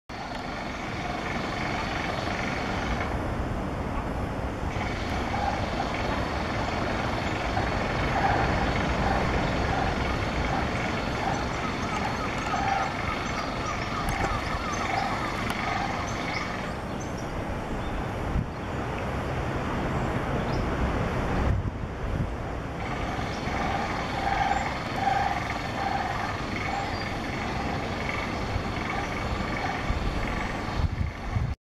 Wall Lizards
15 Mar 18
St Andrew's Church, Church Ope Cove, Portland, Dorset

These 2 Wall Lizards (Podarcis muralis) are making their way out into the warm Spring sun.